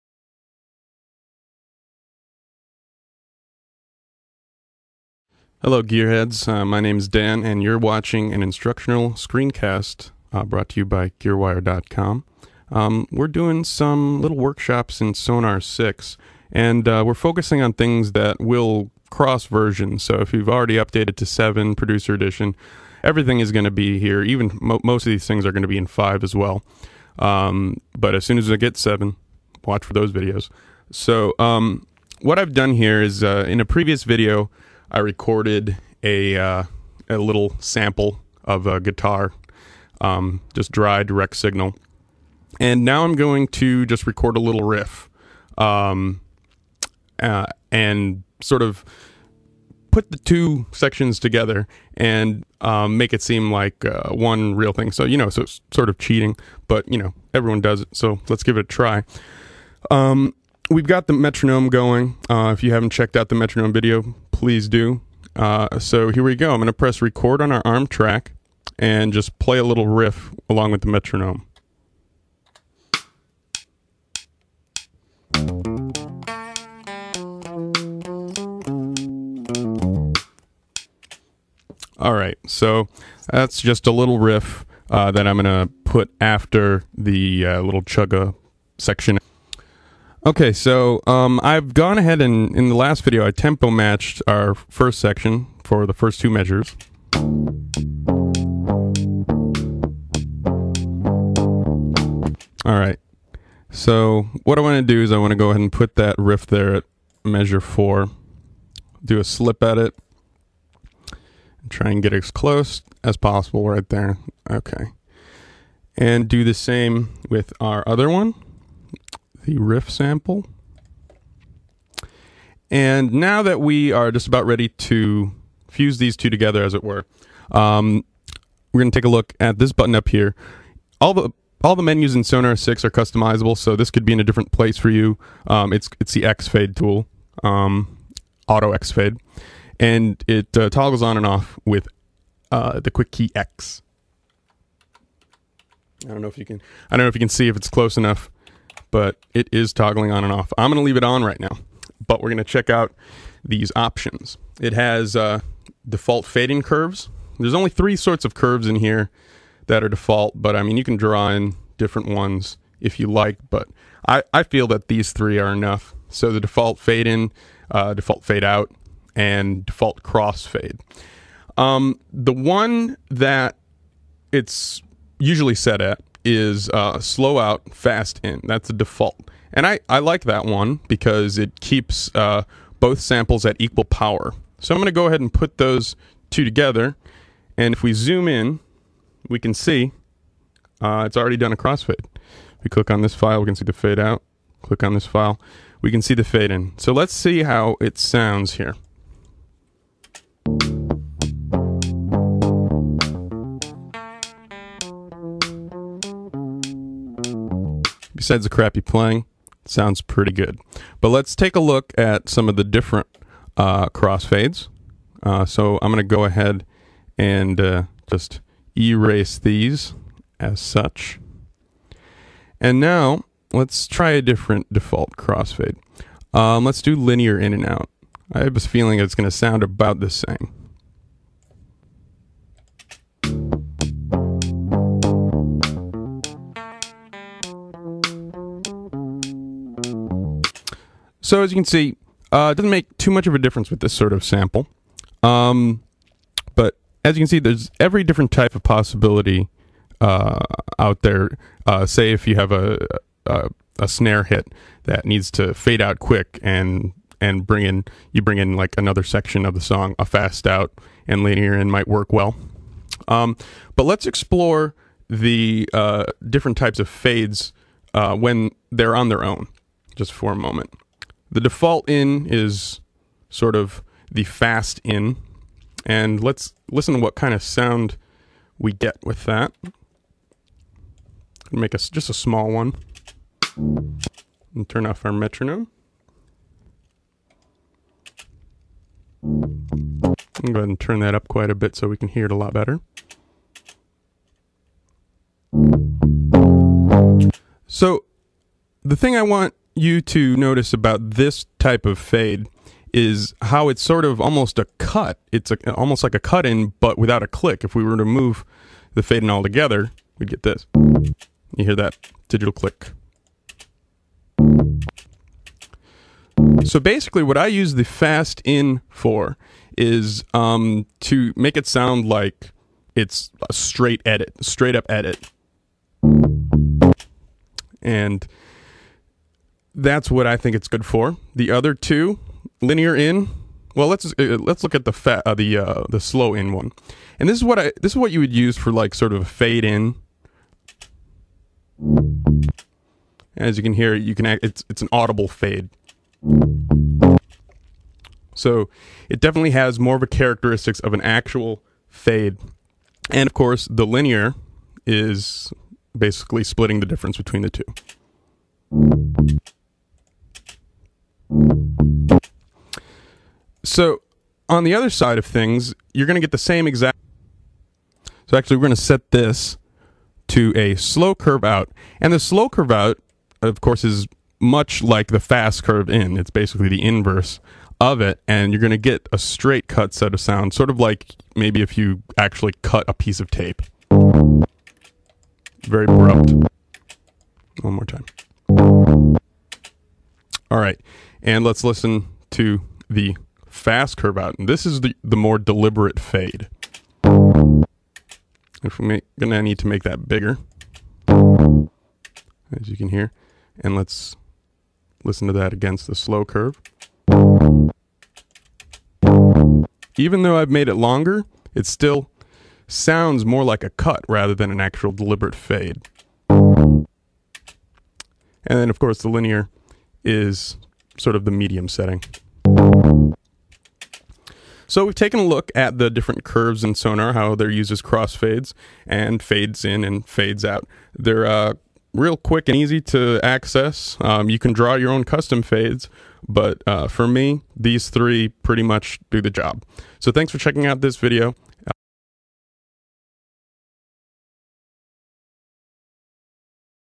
Cakewalk SONAR: Getting Faded With SONAR's Auto X-Fades
Remember when visiting the barber meant a quick going over with an electric razor? (I don't really want to remember that, actually.) I also don't want to remember a time before auto-crossfades (or x-fades as I like to call them). Sonar adopted them a few versions back and it's about time they got some attention.
Even though Cakewalk users will sometimes complain about the lack of more fading options, Sonar does provide some very quick and easy ways to prevent the digital clicks caused by a lack of fading. Even though automatic zero-crossing edits are so common these days it is still worth taking a listen to the different fading options. Wanna talk about smooth? Let's talk about fades.